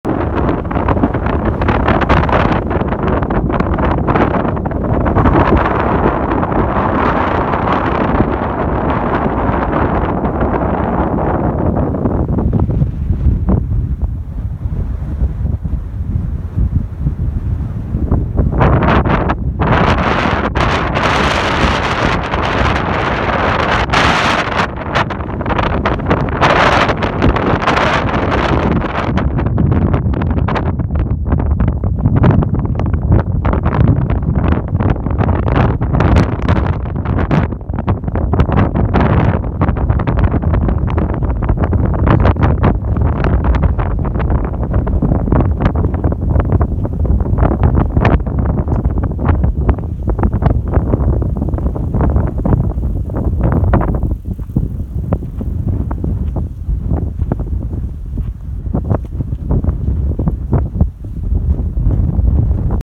Somewhere in the desert
View from a lookout point somewhere in the Bolivian desert.